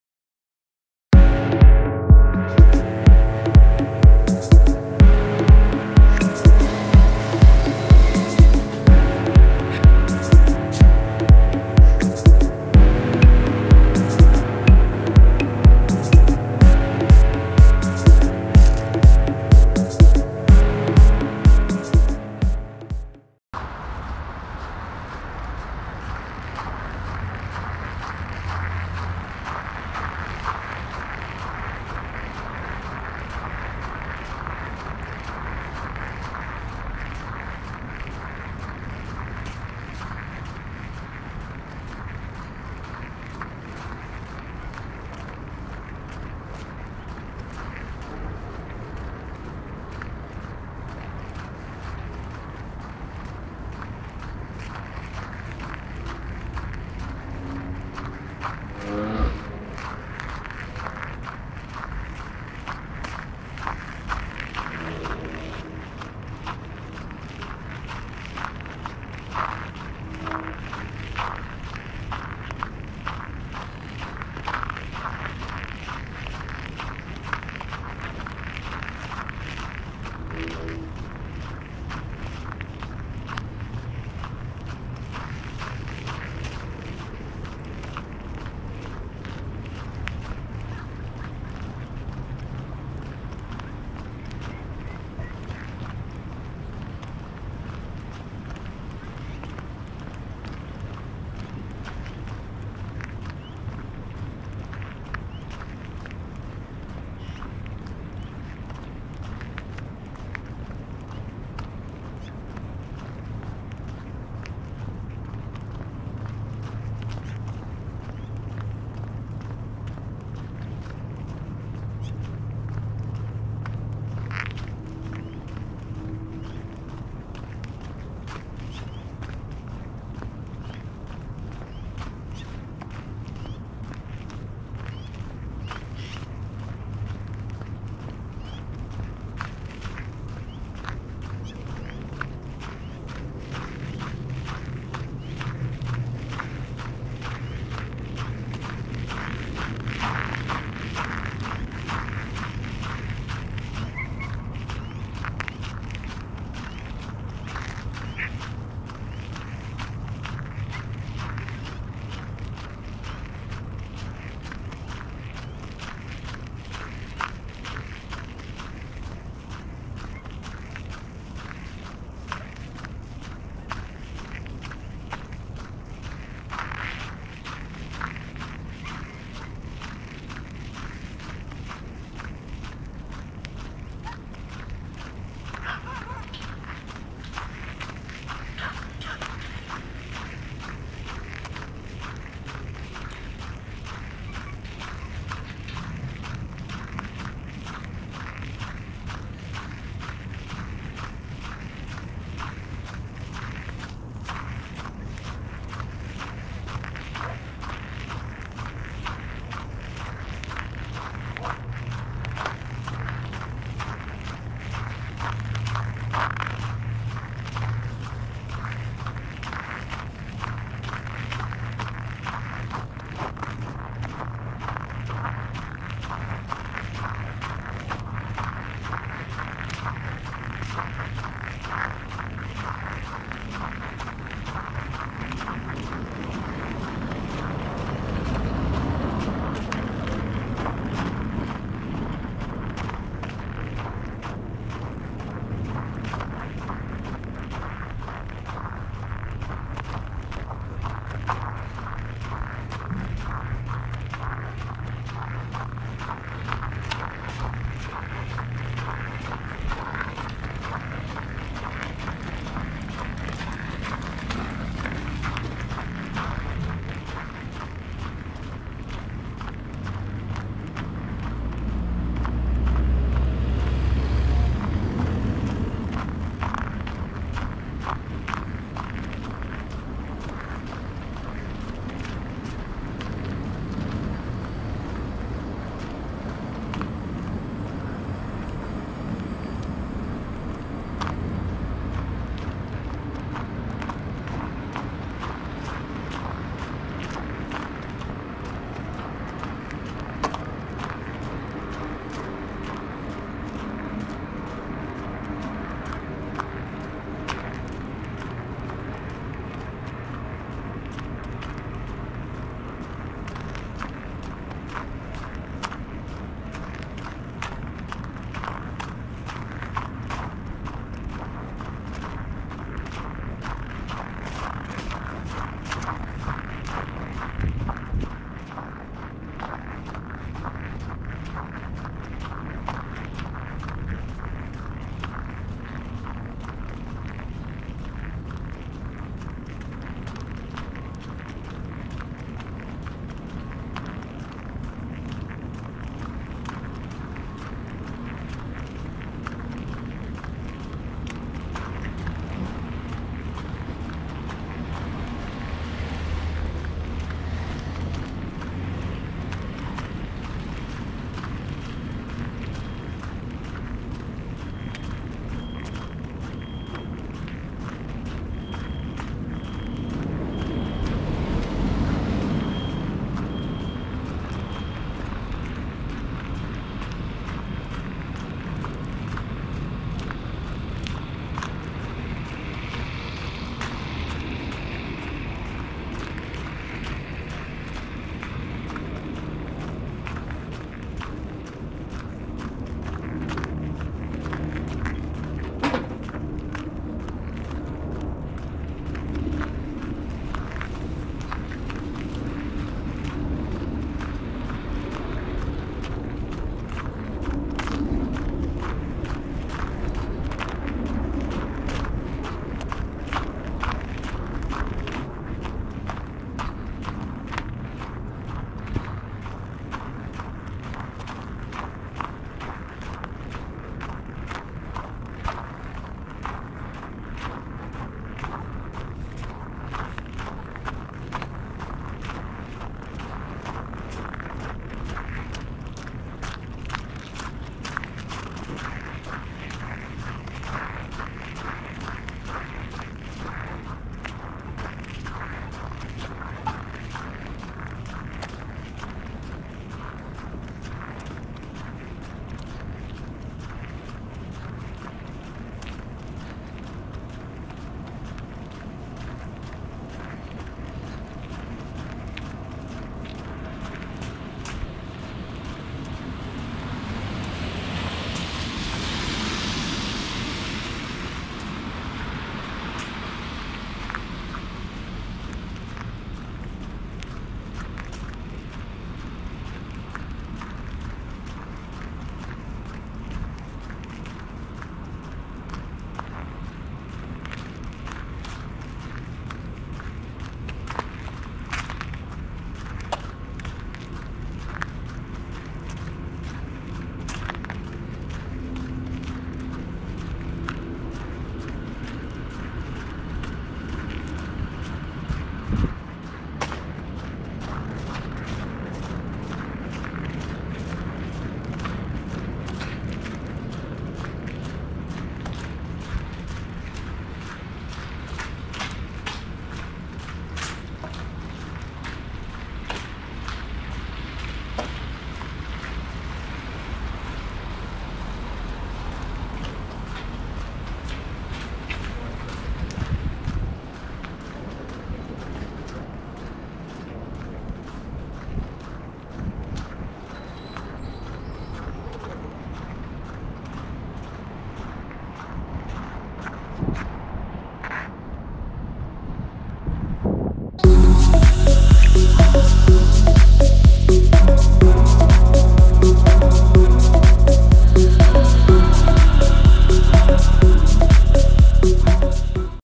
East Village to Downtown
A walk-about from East Village into Downtown.